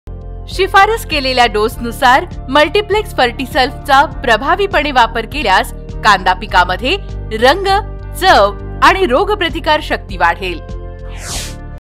Onion Crop Cultivation Tips
शिफारस केलेल्या डोसनुसार मल्टीप्लेक्स फर्टिसल्फचा प्रभावीपणे वापर केल्यास कांदा पिकामध्ये रंग, चव आणि रोग प्रतिकारशक्ती वाढेल.